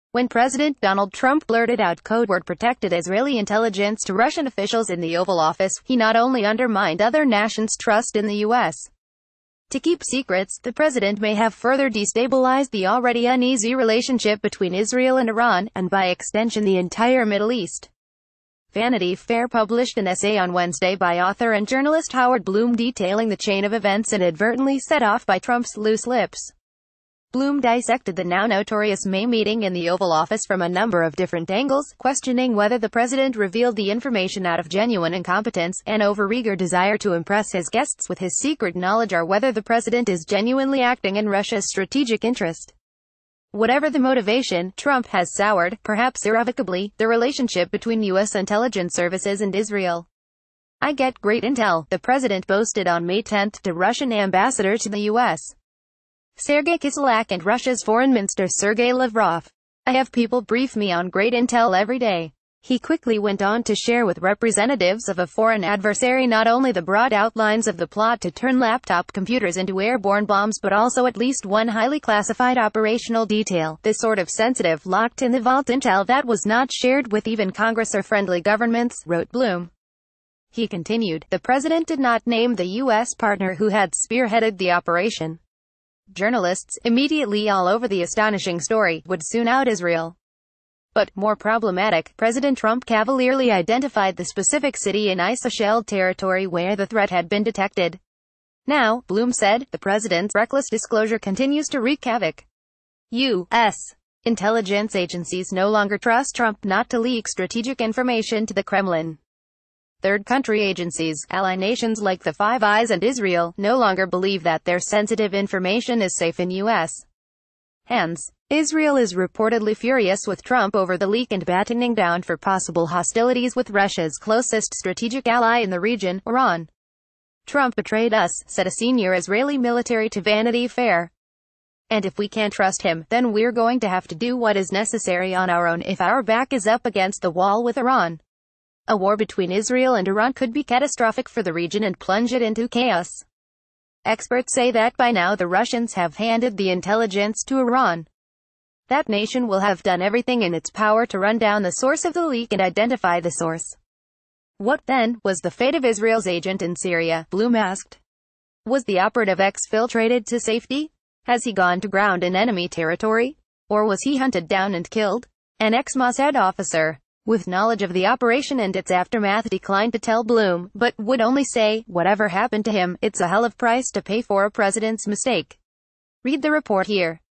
Us Latest News - Trump could have destabilized the entire Middle East by wanting out Israeli intell
Us Latest News - Us Latest News - Trump could have destabilized the entire Middle East by wanting out Israeli intelligence to Russia: report

When President Donald Trump blurted out codeword protected Israeli intelligence to Russian officials in the Oval Office, he not only undermined other nations’ trust in the U.S. to keep secrets, the president may have further destabilized the already uneasy relationship between Israel and Iran — and by extension the entire Middle East. Vanity Fair published an essay on Wednesday by author and journalist Howard Blum detailing the chain of events inadvertently set off by Trump’s loose lips. Blum di...